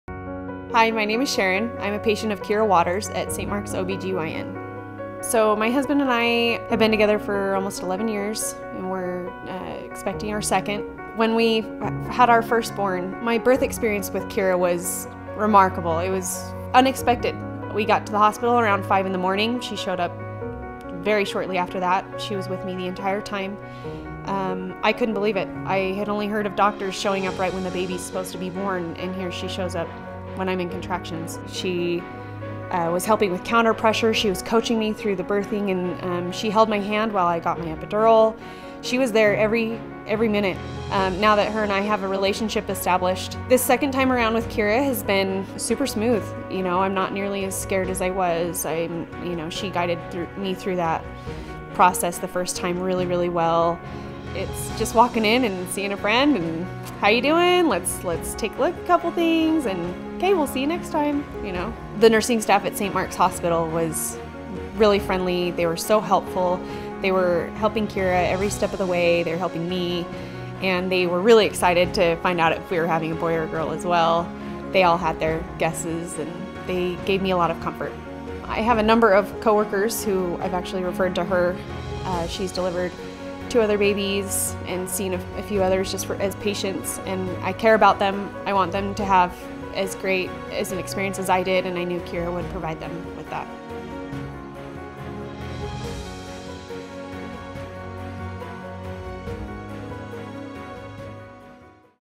Meet Sharon, a patient of Kira Waters, CNM and St. Mark’s OBGYN
Sharon was blown away with how Kira Waters was there during every step of her first pregnancy, and most importantly during her entire delivery. Sharon is now completely confident with Kira and St. Mark’s OBGYN as she prepares for baby number two.